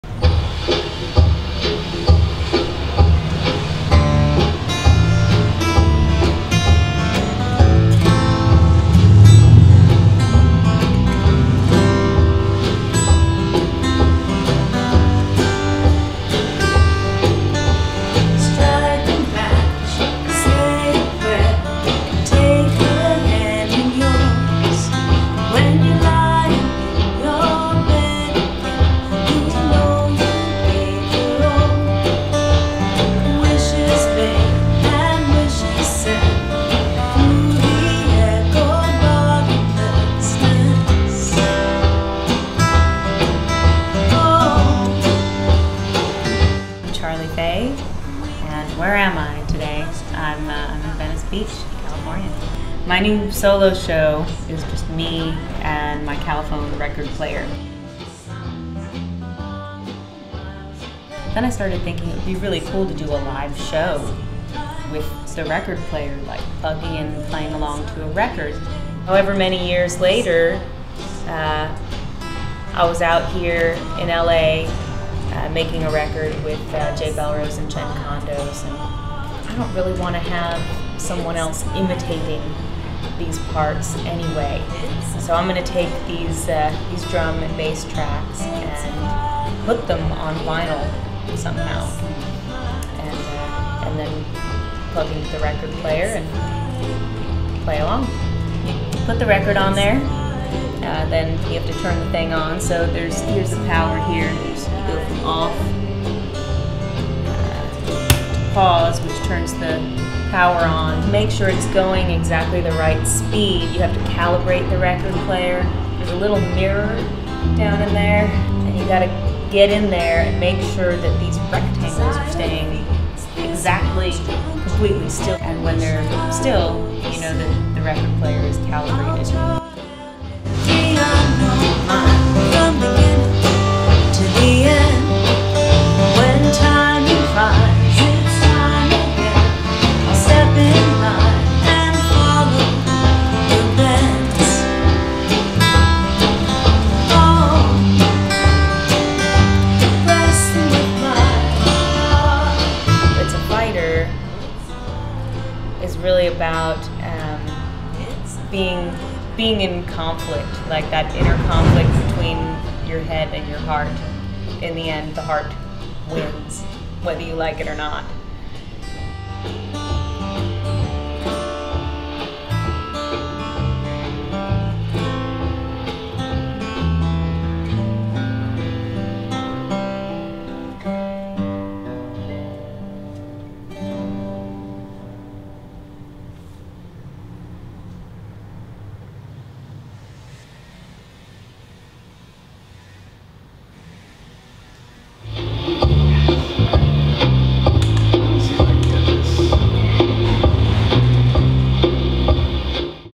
An Afternoon with Charlie Faye (Part 1) // The Bluegrass Situation
Charlie Faye hung out with The Sitch during her recent visit to LA and showed off her unique performance setup.